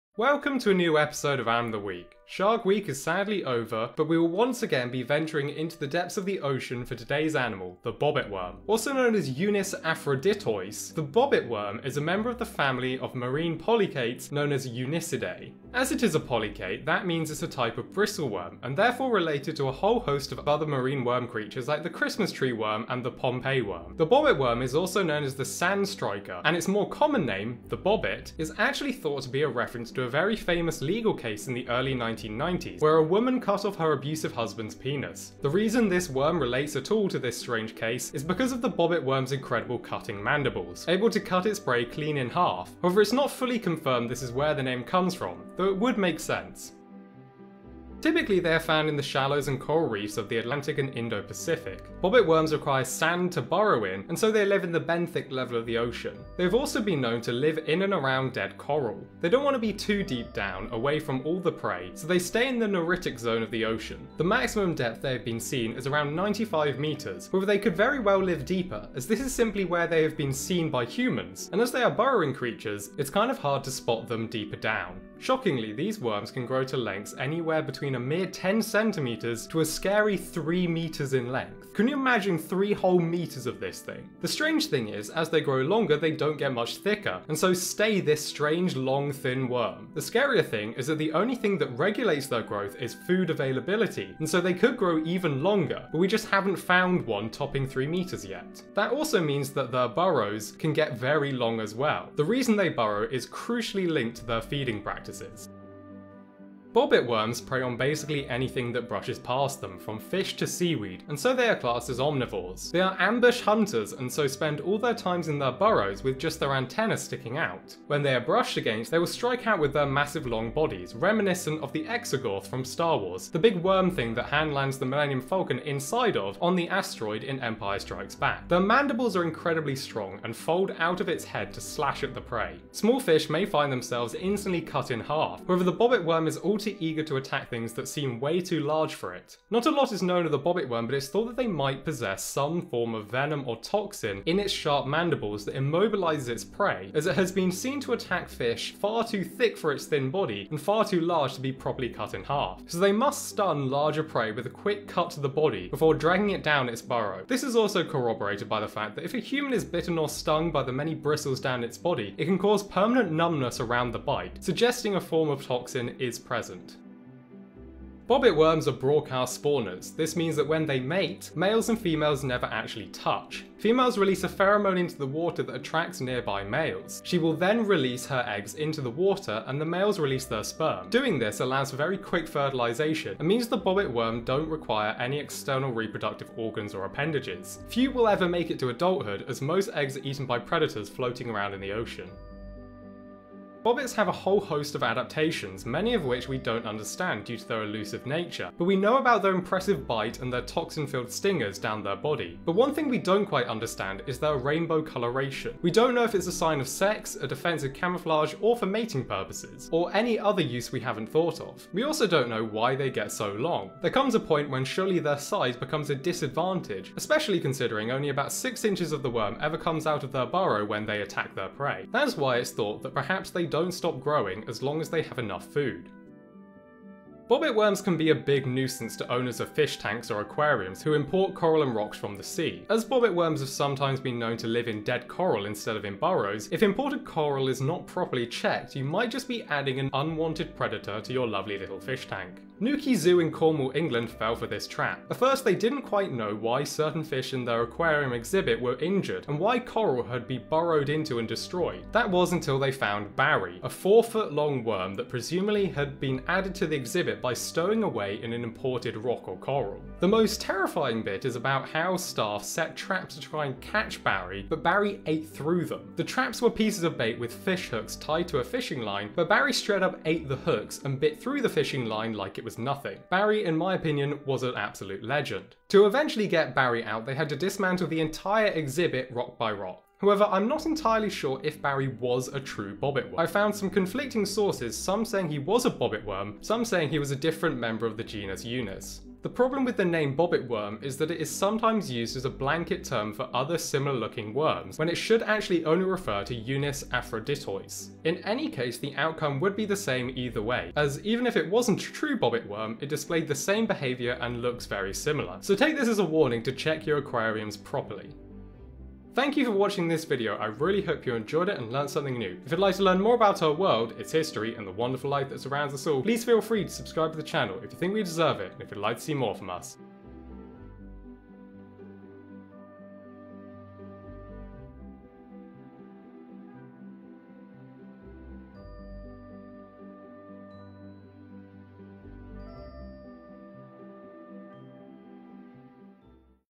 Bobbit Worm - Animal of the Week
This (not worm) week we're looking at the Bobbit Worm, a very strange polychaete that ambushes its prey by burying itself in sediment.